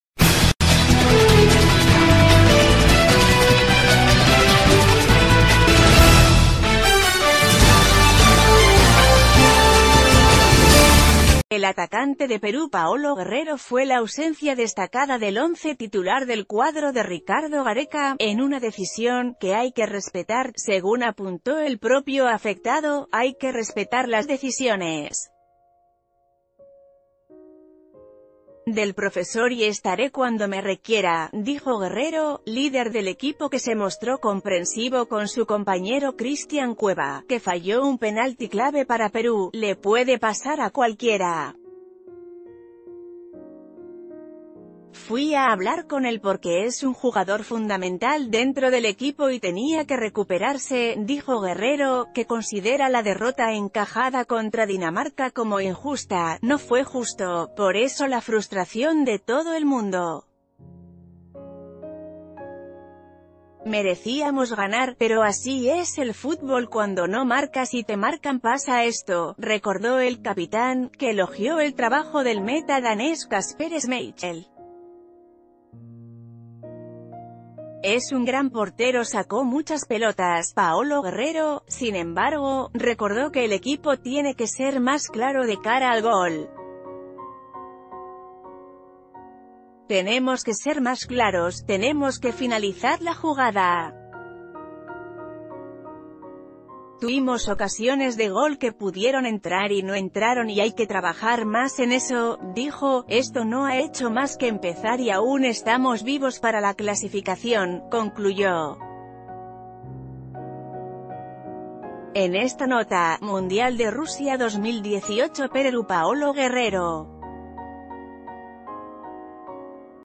¿Por qué Paolo Guerrero no fue titular?
El atacante de Perú Paolo Guerrero fue la ausencia destacada del once titular del cuadro de Ricardo Gareca, en una decisión "que hay que respetar", según apuntó el propio afectado."Hay que respetar las decisiones del profesor y estaré cuando me requiera", dijo Guerrero, líder del equipo que se mostr...